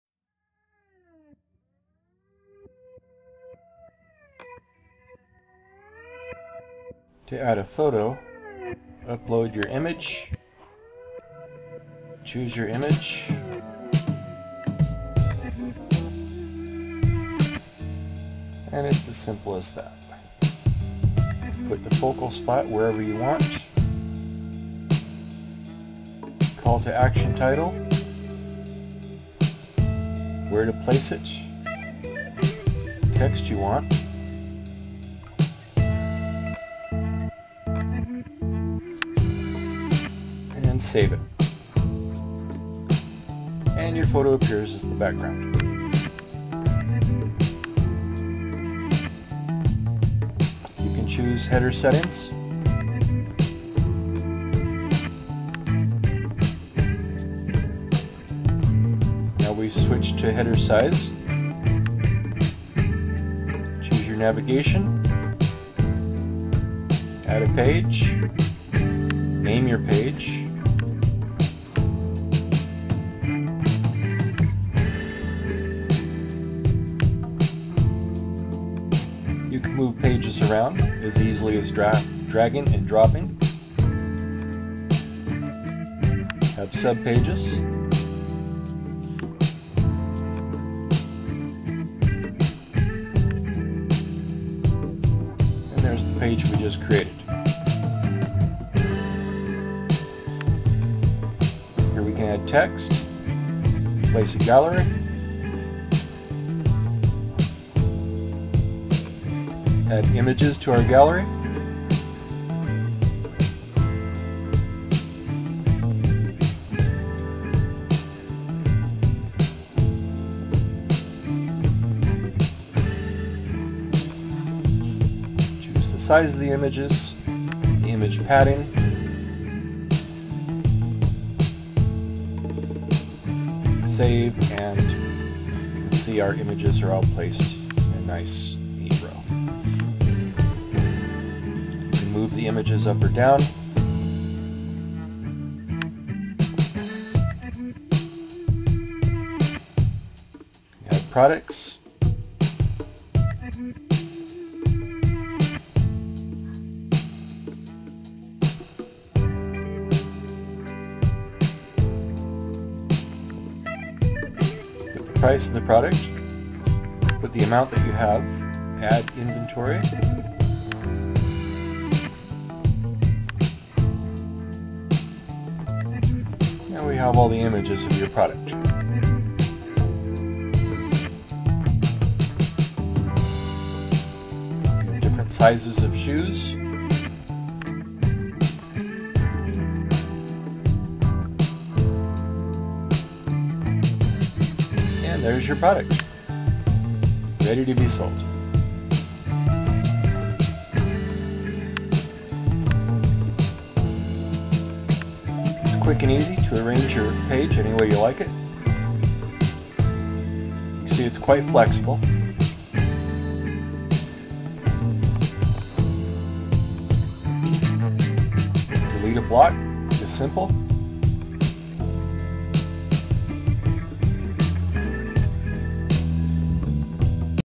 How to create a responsive website on anaZana
anazana.com is the complete e-commerce platform for small businesses and marketing agencies to create smart sites with online stores & sell more online.